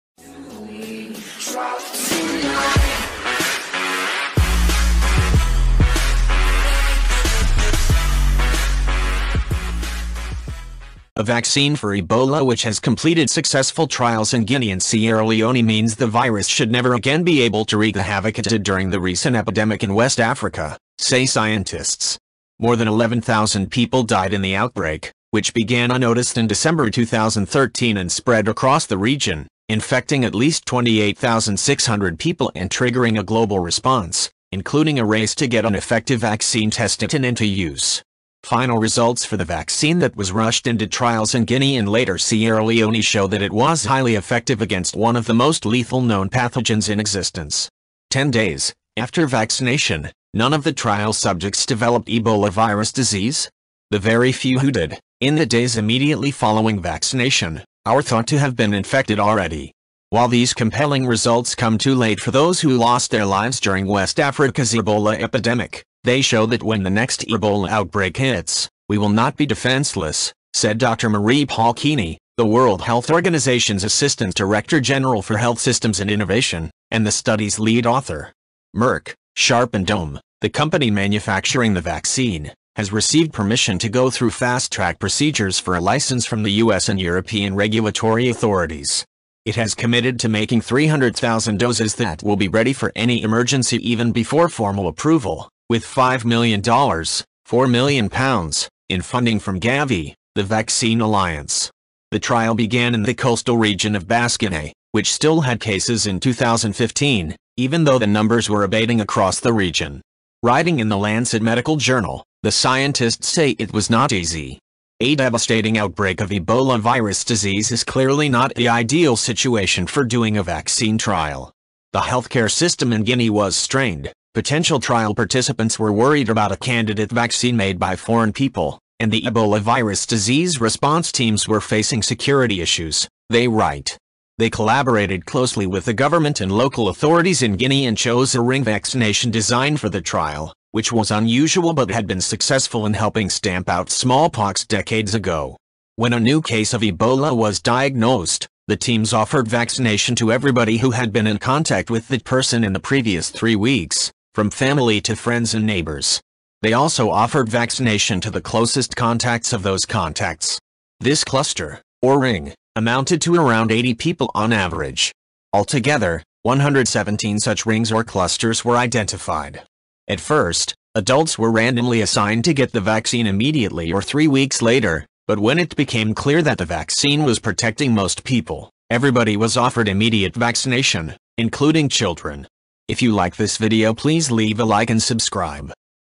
Ebola vaccine is safe and effective, scientists declare after trials
A vaccine for Ebola which has completed successful trials in Guinea and Sierra Leone means the virus should never again be able to wreak the havoc it did during the recent epidemic in west Africa, say scientists.

More than 11,000 people died in the outbreak, which began unnoticed in December 2013 and spread across the region, infecting at least 28,600 people and triggering a global response, including a race to get an effective vaccine tested and into use.

Final results for the vaccine that was rushed into trials in Guinea and later Sierra Leone show that it was highly effective against one of the most lethal known pathogens in existence. Ten days after vaccination, none of the trial subjects developed Ebola virus disease. The very few who did, in the days immediately following vaccination, are thought to have been infected already.

“While these compelling results come too late for those who lost their lives during west Africa’s Ebola epidemic, they show that when the next Ebola outbreak hits, we will not be defenceless,” said Dr Marie-Paule Kieny, the World Health Organisation’s assistant director general for health systems and innovation, and the study’s lead author.Merck, Sharp & Dohme, the company manufacturing the vaccine, has received permission to go through fast-track procedures for a licence from the US and European regulatory authorities. It has committed to making 300,000 doses that will be ready for any emergency even before formal approval, with $5m (£4m) in funding from Gavi, the Vaccine Alliance.

The trial began in the coastal region of Basse-Guinée, which still had cases in 2015, even though the numbers were abating across the region. Writing in the Lancet medical journal, the scientists say it was not easy.

“A devastating outbreak of Ebola virus disease is clearly not the ideal situation for doing a vaccine trial. The healthcare system in Guinea was strained, potential trial participants were worried about a candidate vaccine made by foreign people, and the Ebola virus disease response teams were facing security issues,” they write.

They collaborated closely with the government and local authorities in Guinea and chose a “ring vaccination” design for the trial, which was unusual but had been successful in helping stamp out smallpox decades ago.

When a new case of Ebola was diagnosed, the teams offered vaccination to everybody who had been in contact with that person in the previous three weeks, from family to friends and neighbours. They also offered vaccination to the closest contacts of those contacts. This cluster – or ring – amounted to around 80 people on average. Altogether, 117 such rings or clusters were identified. At first, adults were randomly assigned to get the vaccine immediately or three weeks later, but when it became clear that the vaccine was protecting most people, everybody was offered immediate vaccination, including children.